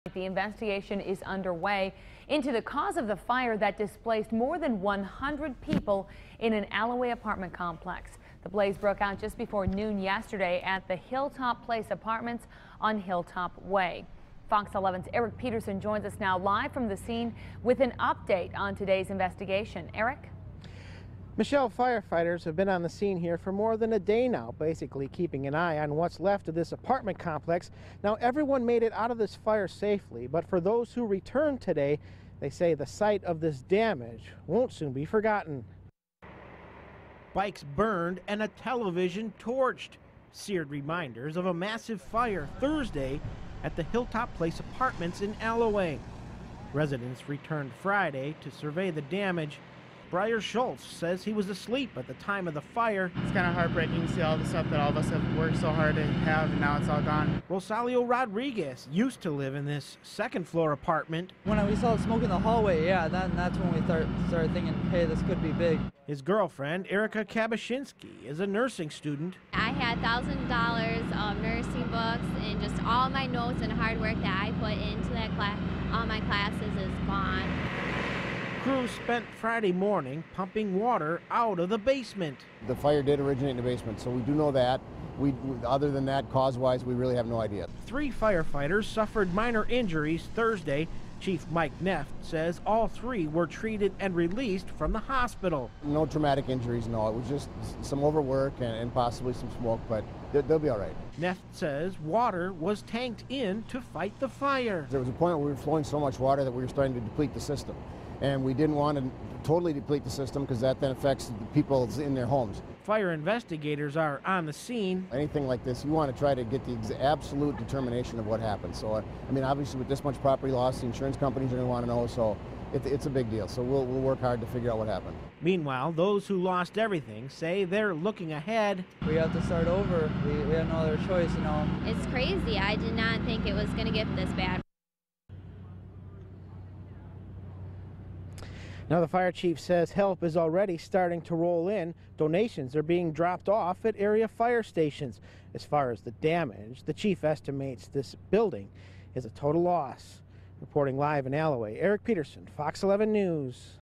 5PM FRI ALLOUEZ FIRE LATEST
The investigation is underway into the cause of fire that displaced more than 100 people in an Allouez apartment complex.